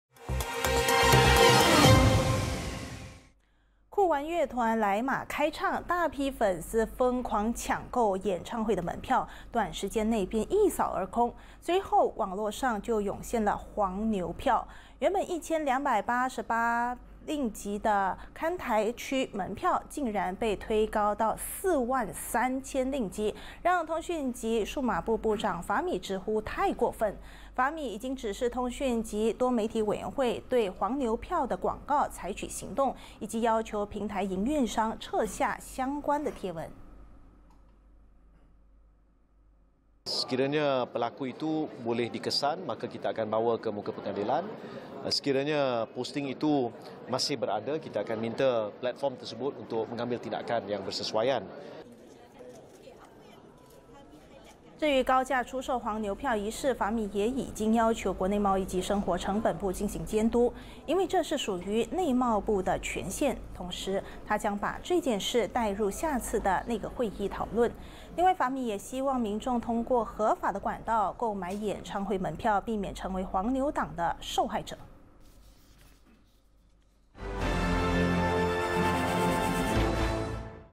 20230518 酷玩黄牛票炒至4.3万 法米指示MCMC采取行动
酷玩乐团来马开唱，大批粉丝疯狂抢购演唱会门票，短时间内便一扫而空。随后网络上就涌现了黄牛票。原本1千288令吉的看台区门票，竟然被推高至4万3千令吉，让通讯及数码部部长法米直呼太过分。对此，法米已经指示通讯及多媒体委员会，对黄牛票的广告采取行动，以及要求平台营运商撤下相关的贴文。